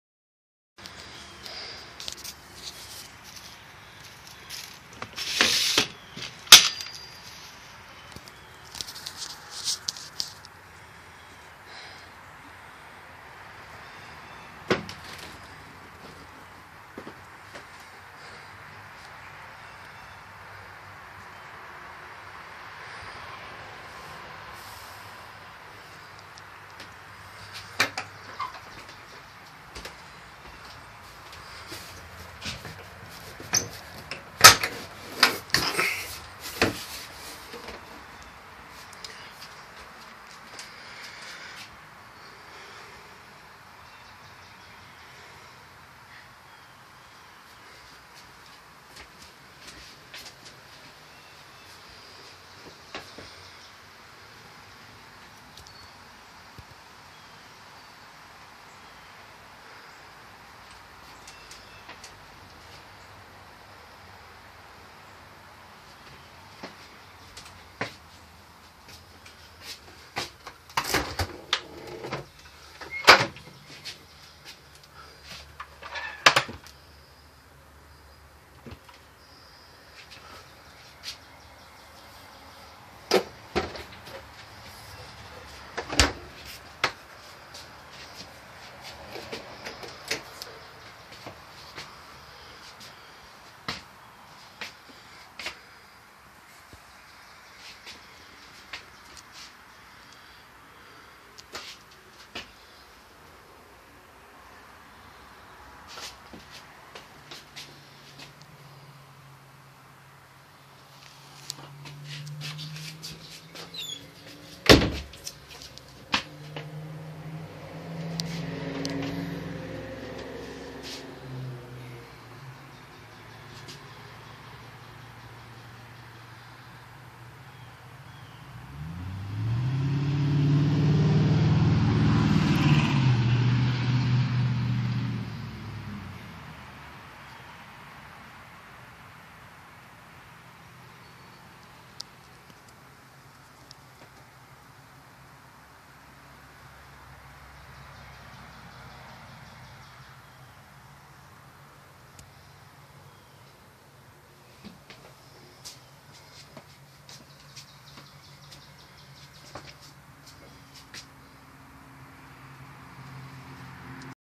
92 Scotty camper before remodeling
We picked up this camper from a friend and stripped the wallpaper and rebuilt one front corner of it.